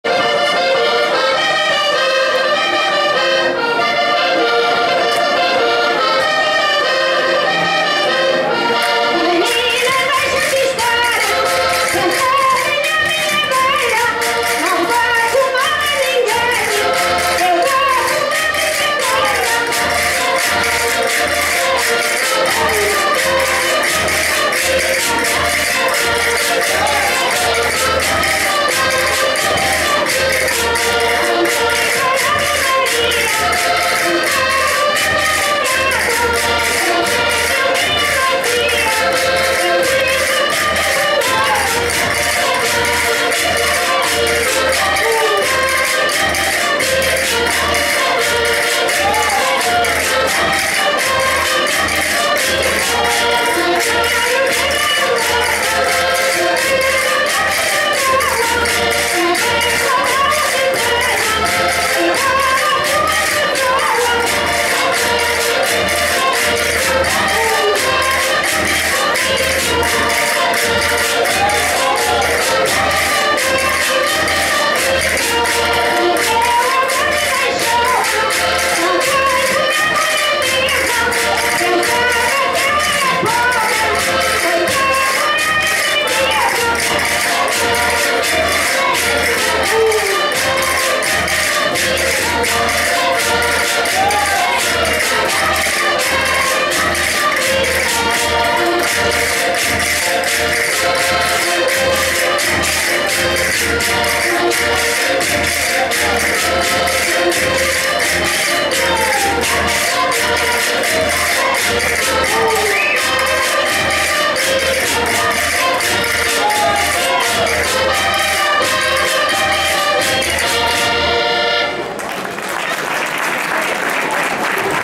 Festival De St Michel Sur Orge le 10/02/2013.
Filmé & Mise en Ligne par Rose Da Silva .

- L'association Franco-Portugaise de St Michel sur orge : Tradiçoes Do Alto Minho